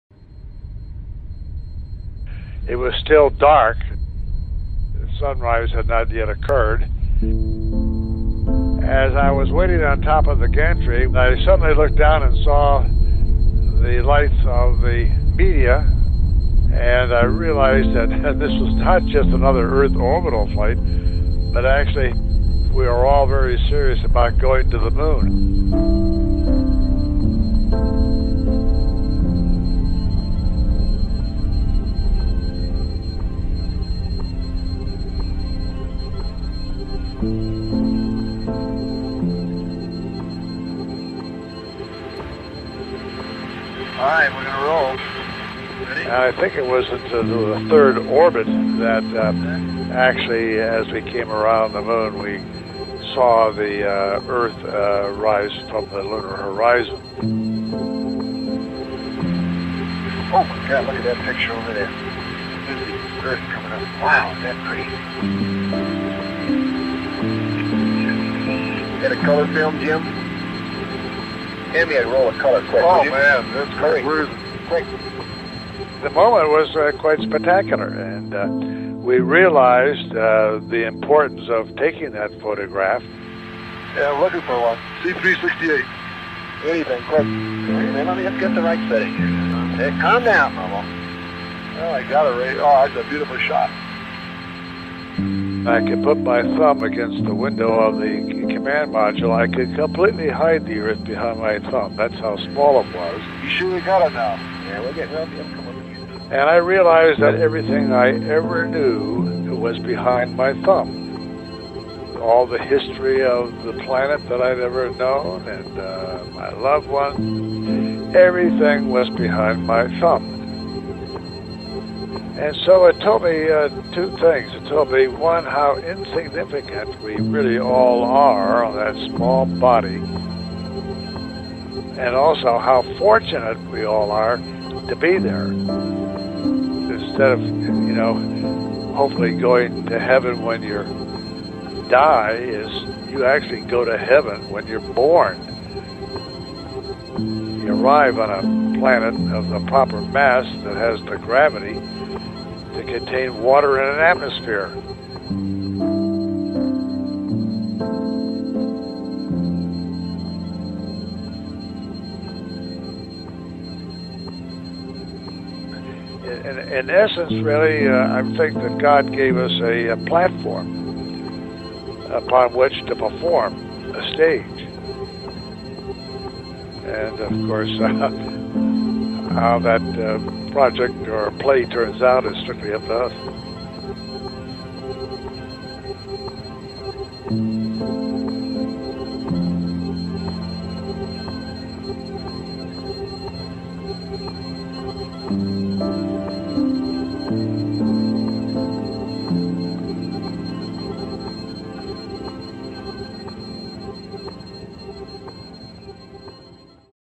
Apollo 8 and the Story of Earthrise
A short film meditation on the famous Earthrise photo - the first photo of the whole earth ever taken by humans. Featuring Apollo 8 Astronaut Jim Lovell, and footage from the Apollo missions.

Made for the new season of The Elephant, a podcast investigating solutions to the greatest problem of our time: climate change.